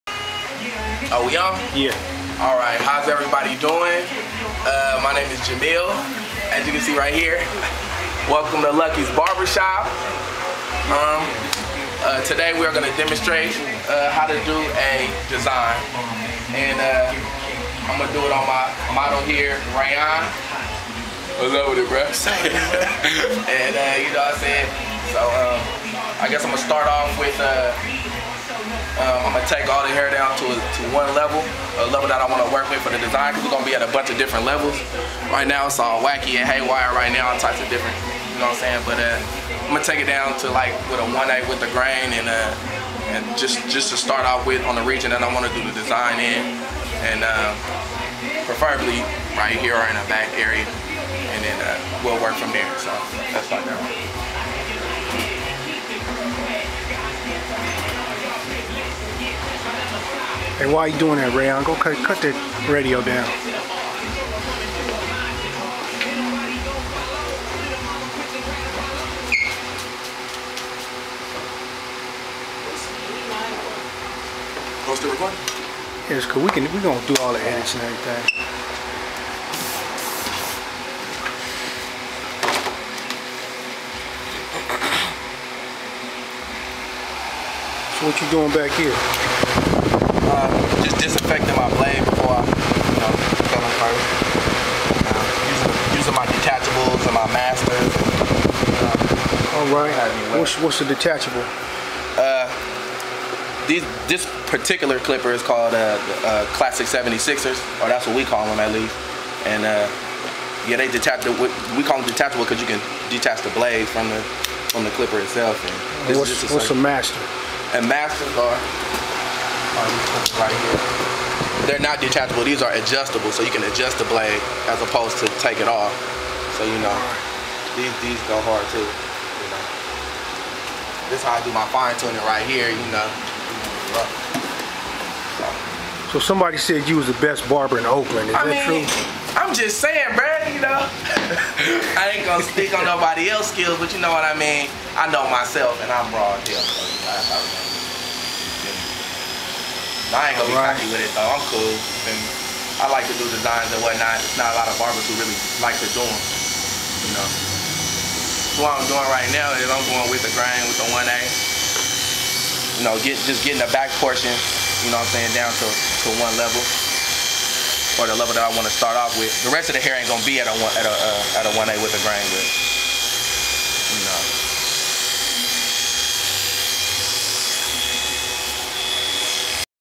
Jamil demonstrates haircut with design
Jamil of Lucky's Barbershop in Oakland CA demonstrates eleaborate designs.